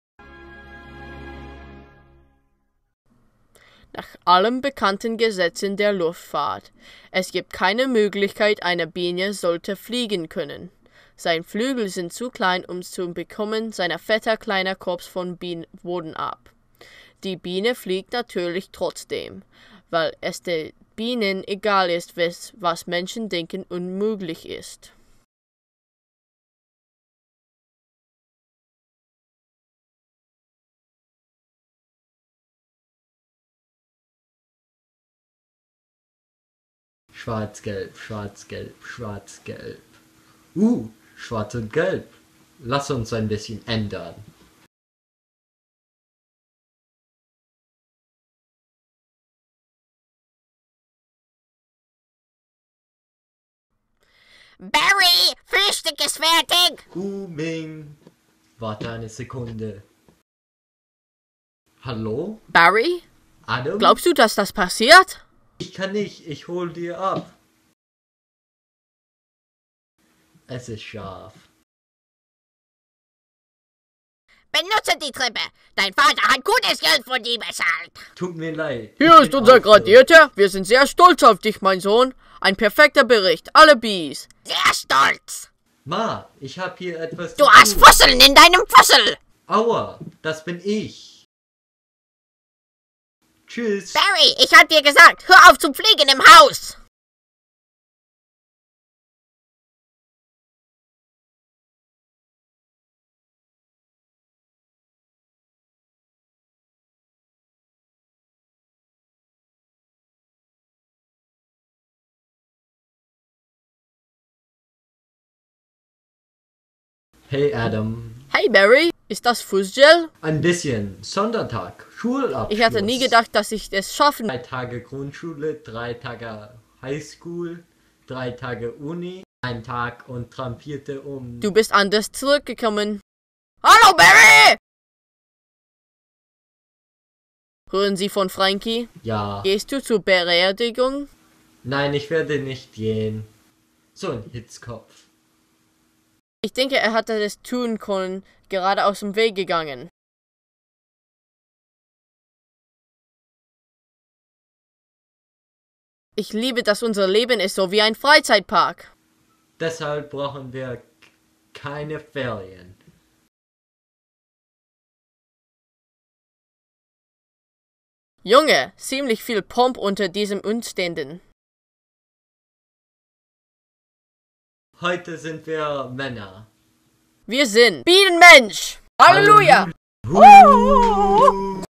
The Bee Movie but it's my German Project
I made this for a german project with my friend BeBbBeBBe Mebz(as pictured in diagram 47A) who appears in the video as barry B bensong